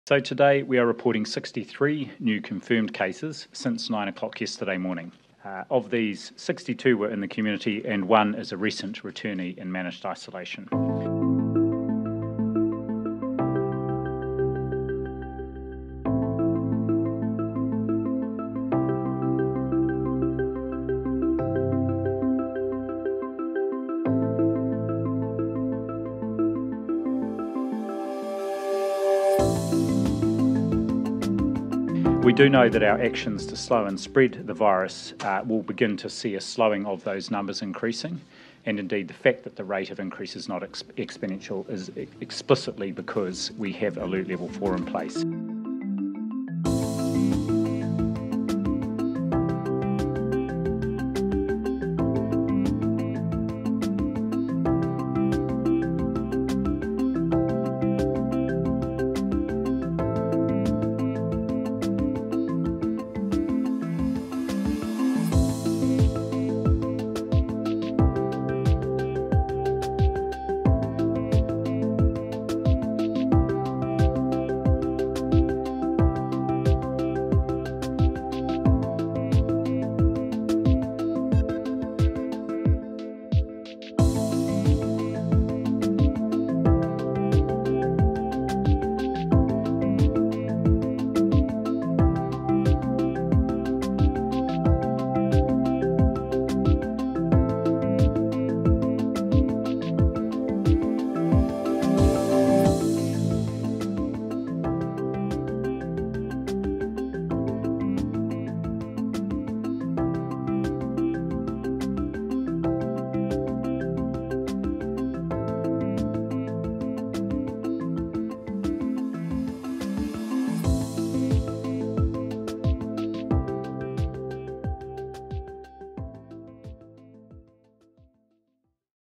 NZ Delta outbreak | Wednesday 25th Aug Wrap | nzherald.co.nz
A daily wrap of all the main developments in the Covid-19 Delta outbreak as New Zealand is plunged into lockdown for the fifth time.